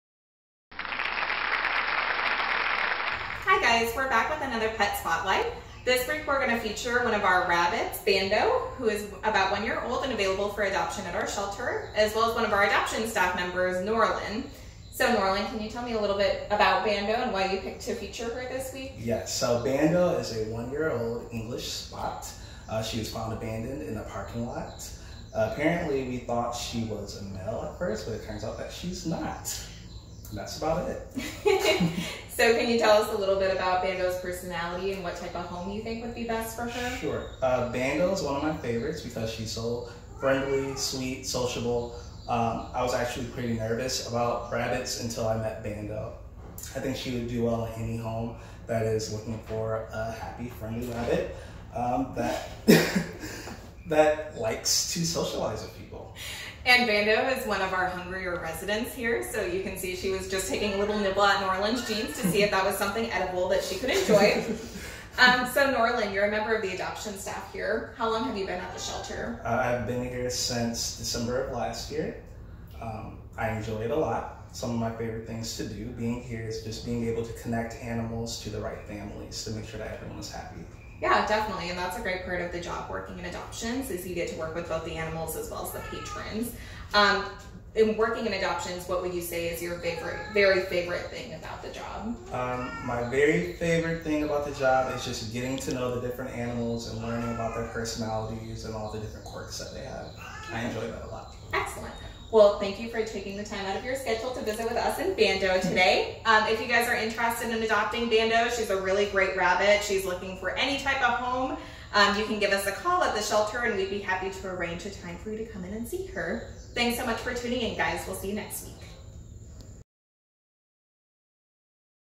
Pet Spotlight (ft. Bando)
For the love of critters; come check out our gorgeous girl, Bando! This social, one year old bunny rabbit, is patiently waiting for her forever family. She can be found lounging on the top shelf of her kennel or up at the front hoping to convince passers by for some snacks. Tune in as Norlan describes this fun companion!